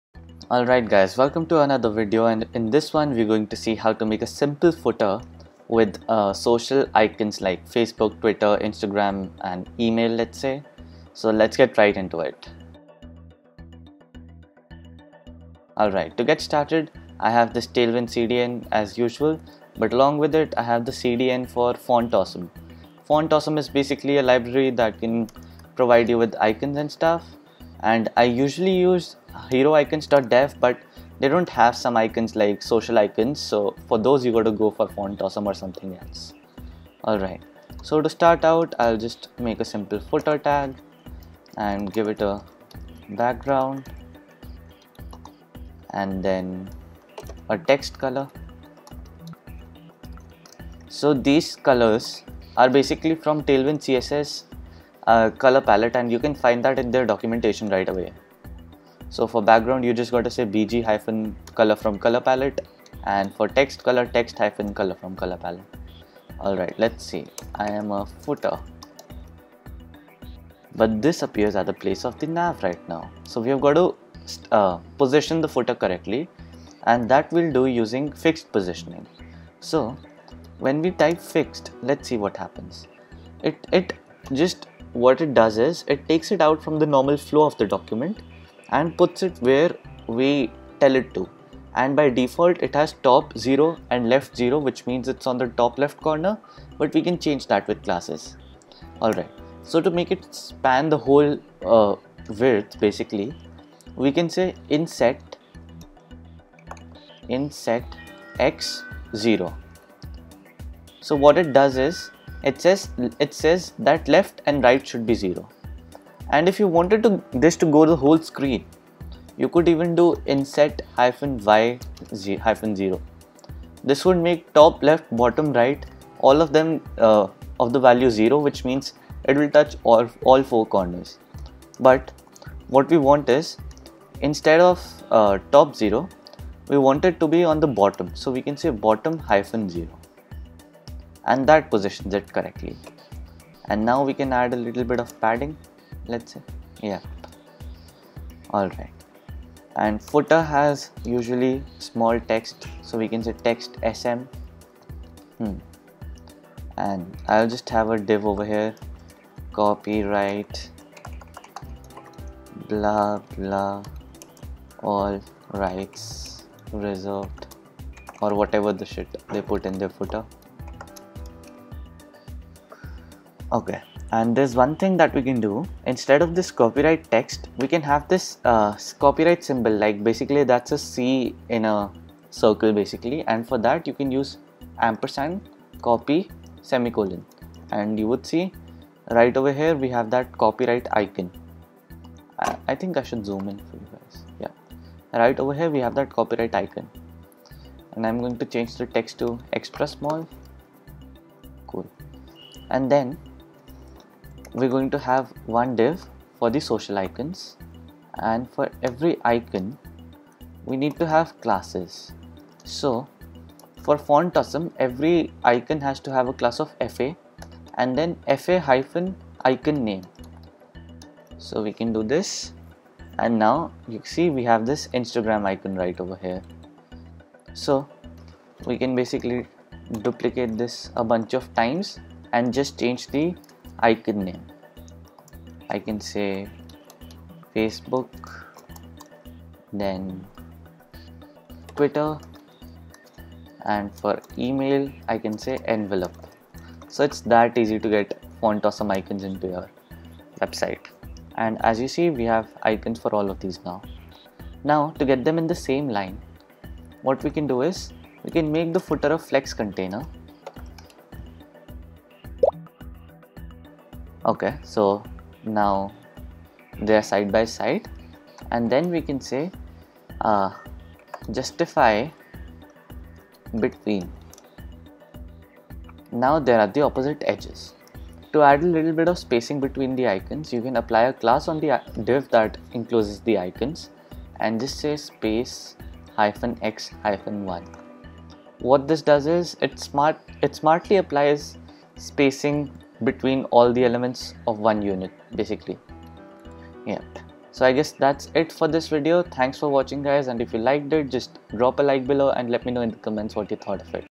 Footer in tailwindcss, EP - 15 | Learn Tailwindcss
Hey guys in this one we're going to take a look at how to make a footer with tailwindcss!